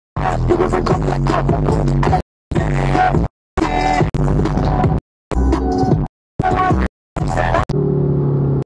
How to fast forward and rewind on tiktoks
This is how to fast forward and rewind on tiktoks so you don’t have to waste time waiting for the last part